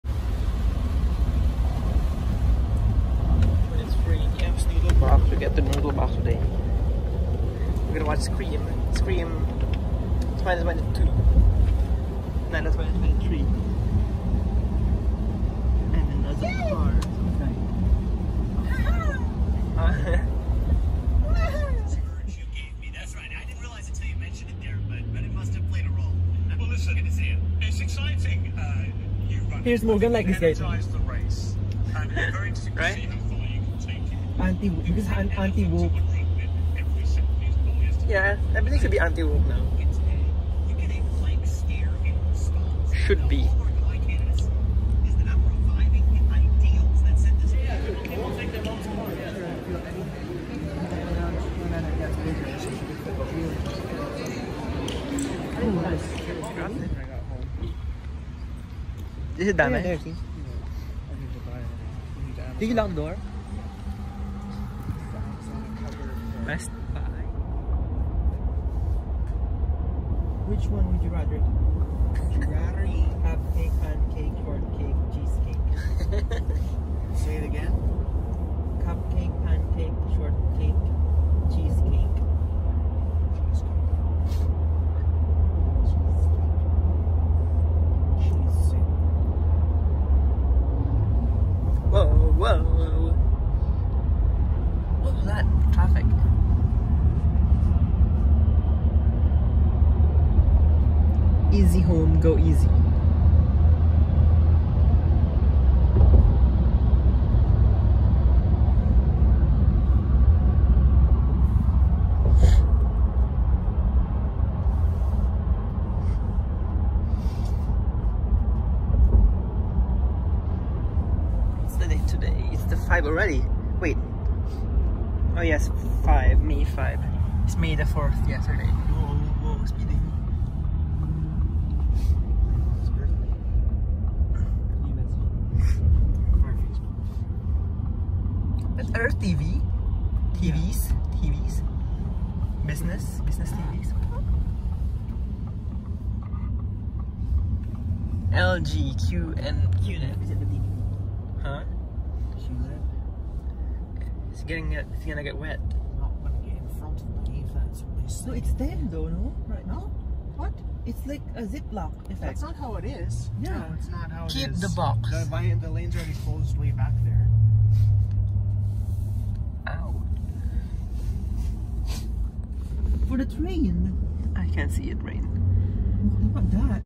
May 5, 2023 - It’s Friyay!
We barely walk on weekdays, we sit down working all week. We’re currently trying to go out more to help our mental health. We went to the mall after work then ordered some noodle box on our way home. We watched Scream 2022.